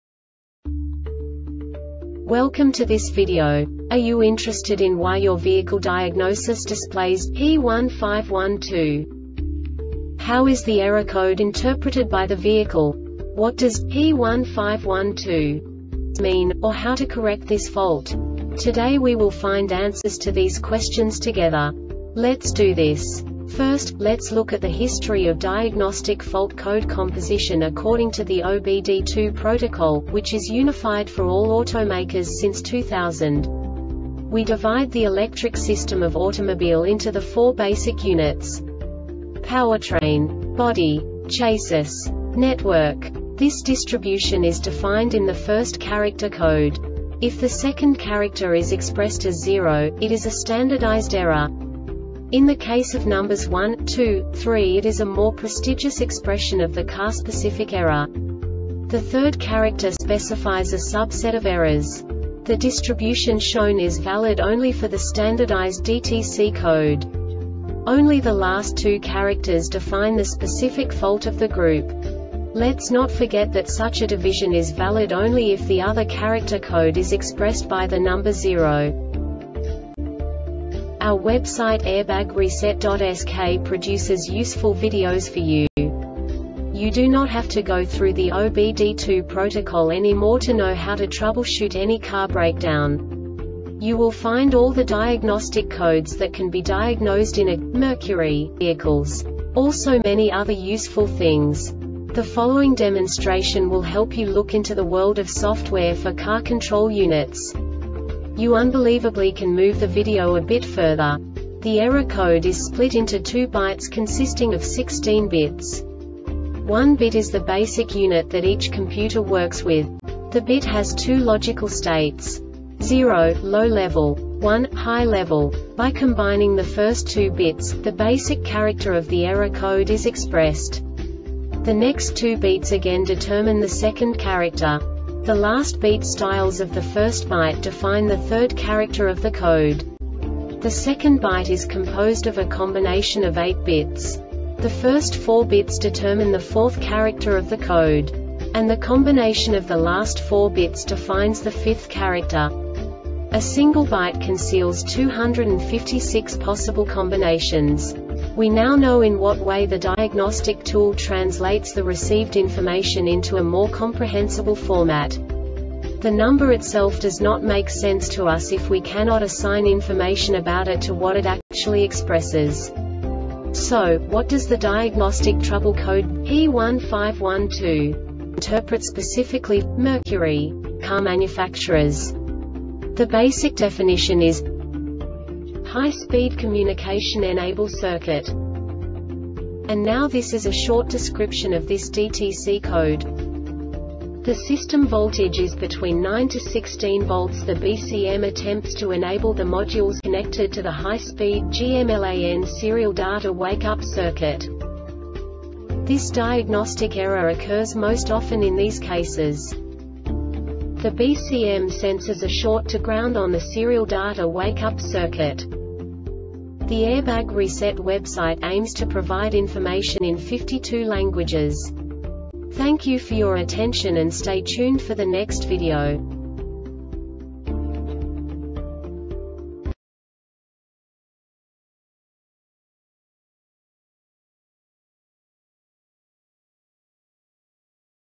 DTC Mercury P1512 Short Explanation
Contents:
0:21 Basic DTC analysis according to OBD2 protocol standard.
1:48 Insight into programming
2:58 A diagnostically understandable description of the Diagnostic Trouble Code
3:05 Basic error definition

Make
Mercury
Code
P1512
Definition
High Speed Communication Enable Circuit
Description
The system voltage is between 9-16 volts  The BCM attempts to enable the modules connected to the high speed GMLAN serial data wake up circuit
Cause
The BCM senses a short to ground on the serial data wake up circuit